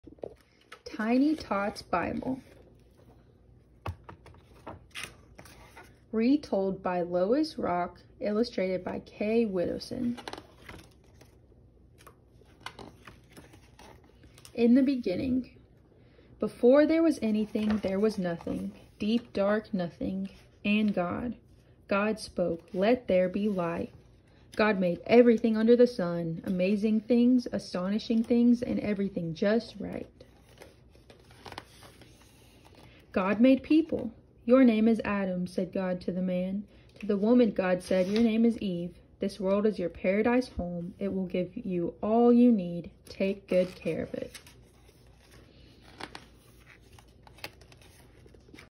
Kid’s Bible Reading- Adam and Eve
Kids Bible Reading- Adam and Eve
Tiny Tots Bible retold by Lois Rock; illustrated by Kay Widdowson

Hi Ya’ll my name is Kameron! I am a Christian woman who is also a mom to a toddler. I love the beach and to travel.